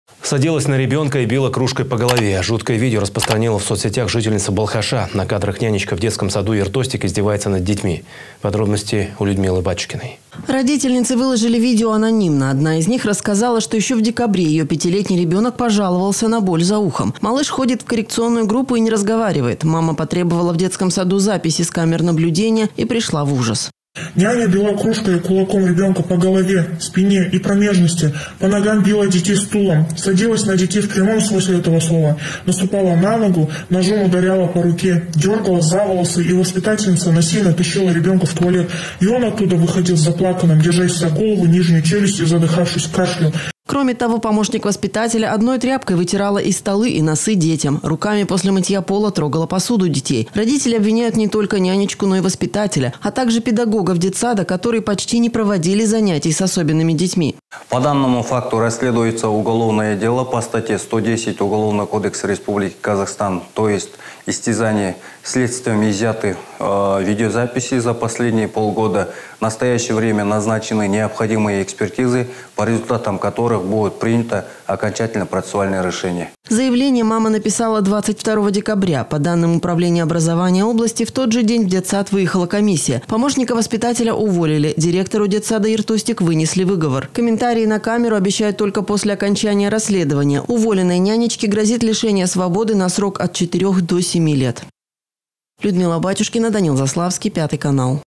Садилась на ребенка и била кружкой по голове.
На кадрах нянечка в детском саду «Ер Тостик» издевается над детьми.